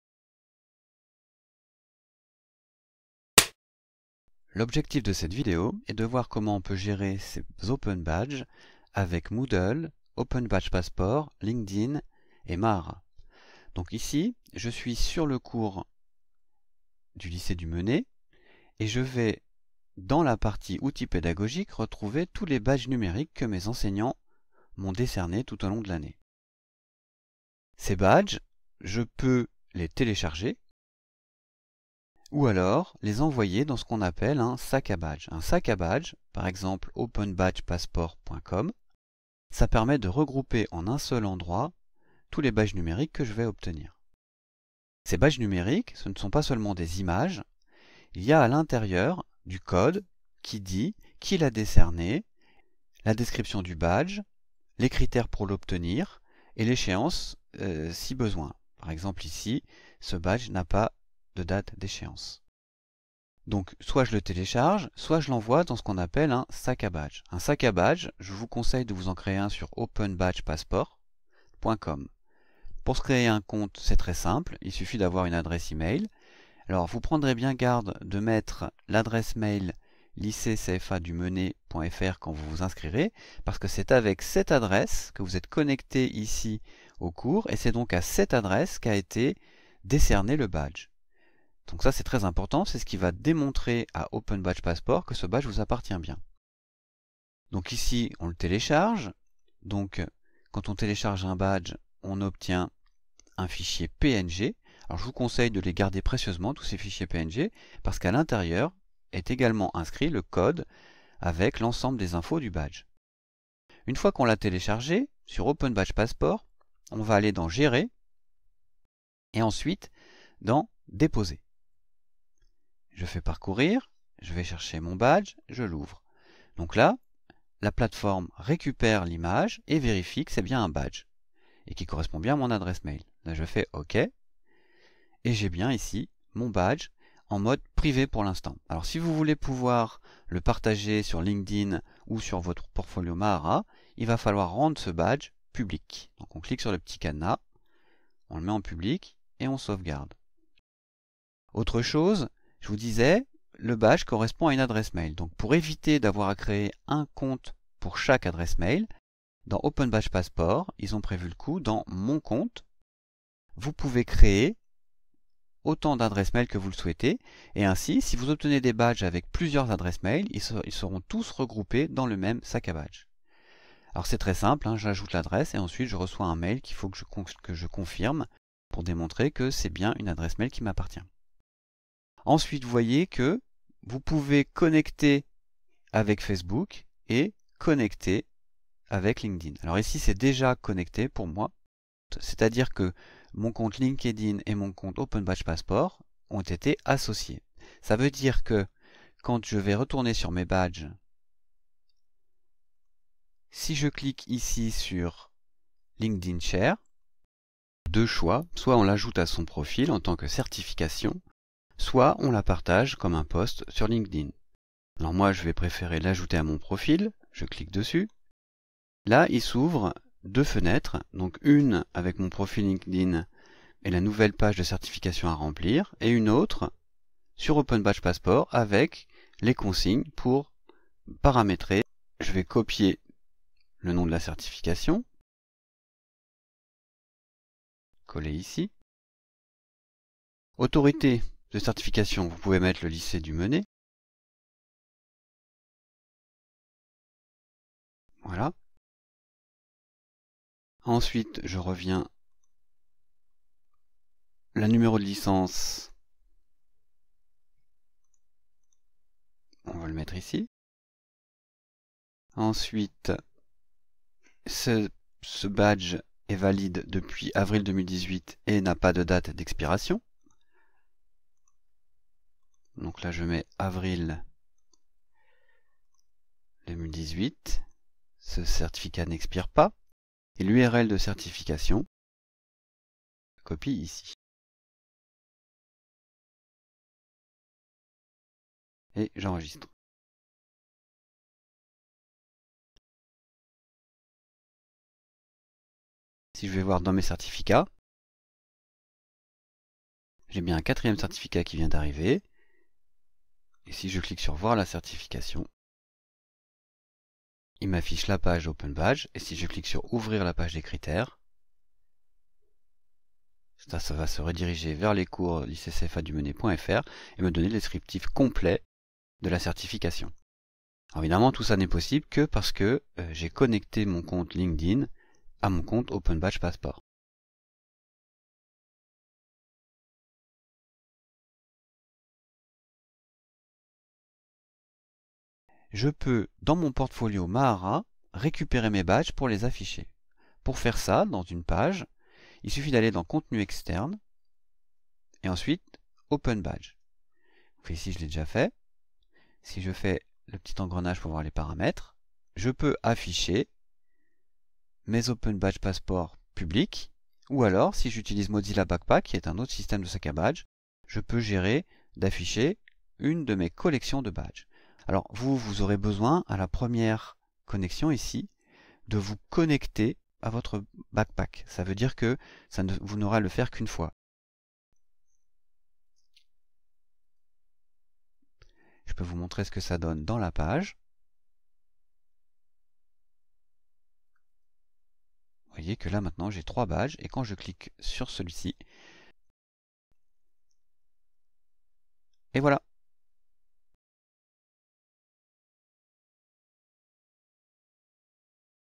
Tuto vidéo : Gérer ses badges numériques (Moodle, Mahara, OpenBadgePassport, LinkedIn)
Points à retenir :
- Dans Moodle : Outils pédagogiques - Badges numériques
- LinkedIn Share - Certification
- Dans Mahara : Contenu externe - Open Badges